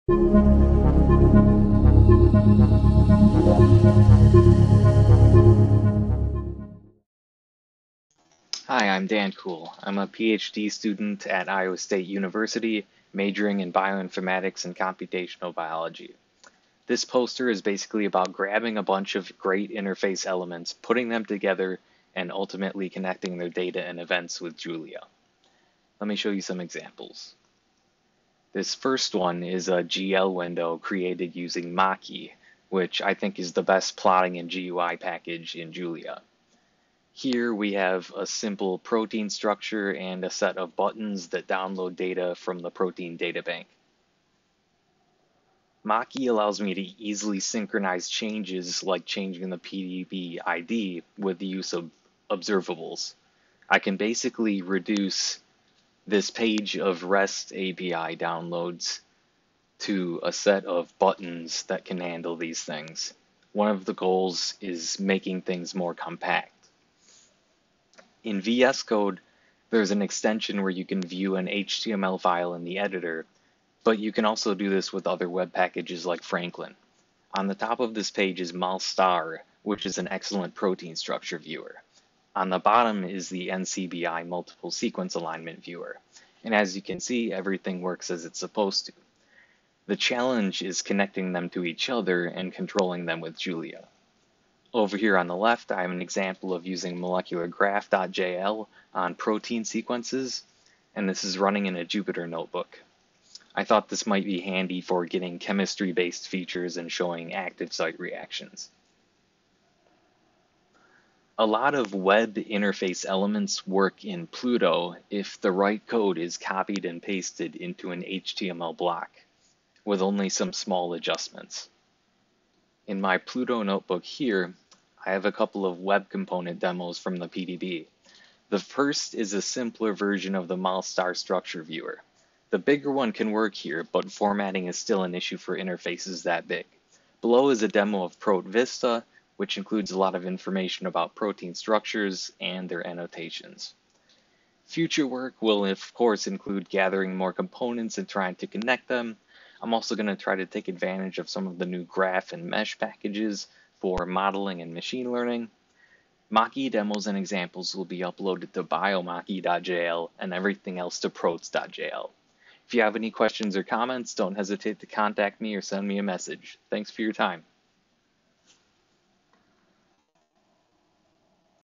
Composite Interfaces for Bioinformatics | Daniel Kool | JuliaCon2021
This poster was presented at JuliaCon2021.

Abstract:
There are plenty of user interface elements for bioinformatics available on the web, but most of them are dispersed and written in JavaScript. With Julia, we can adapt and augment these elements to make interfaces that are more relevant to user projects, have denser information, can be run/tweaked locally, and create new connections between data.

00:00 Welcome!
00:10 Help us add time stamps for this video! See the description for details.